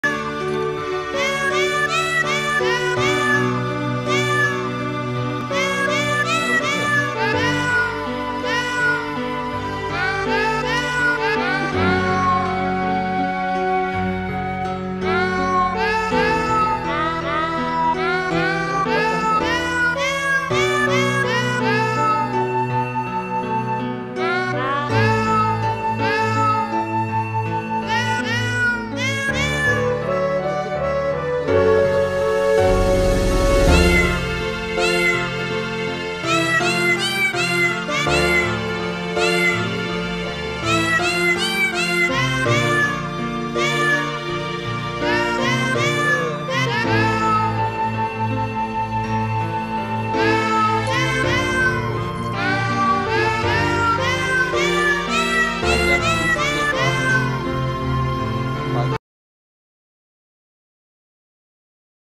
Kucing Hati senang perut kenyang
MintGemuk bersantai dihujung minggu. Hati senang bila perut kenyang dengan Ayam